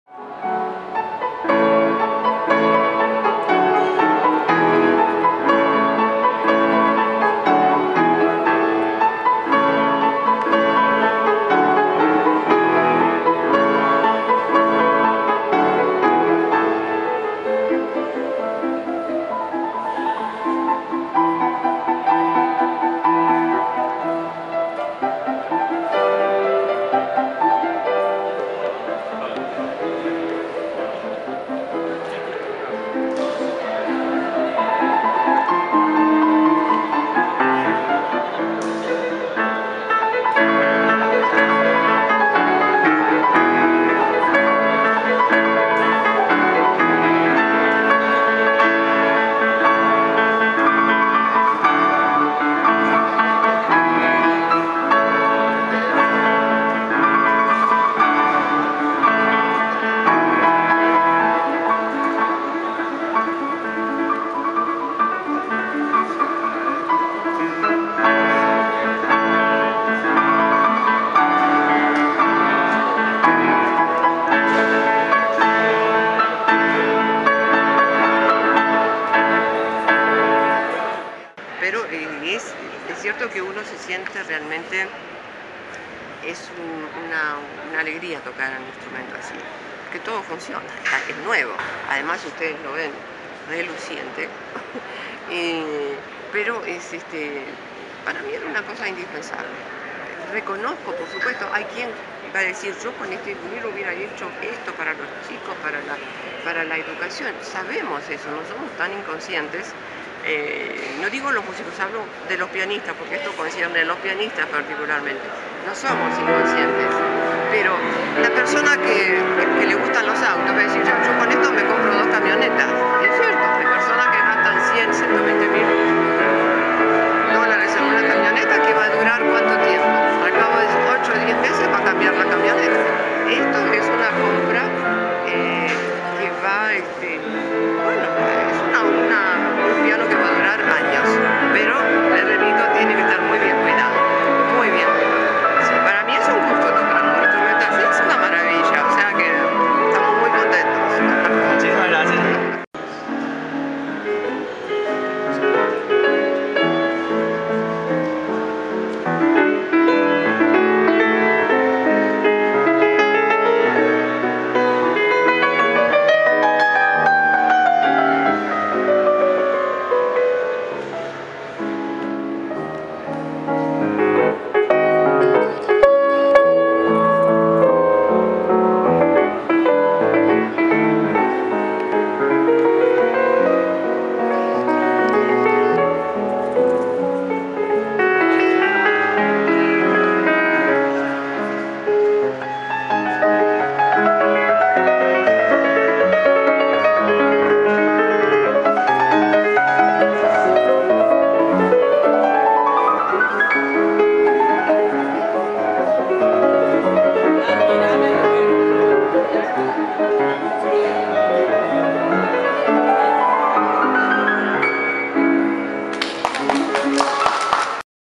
El piano del Congreso - Ultimahora.com
Así suena el piano Steinway & Sons (USA) del Congreso Nacional.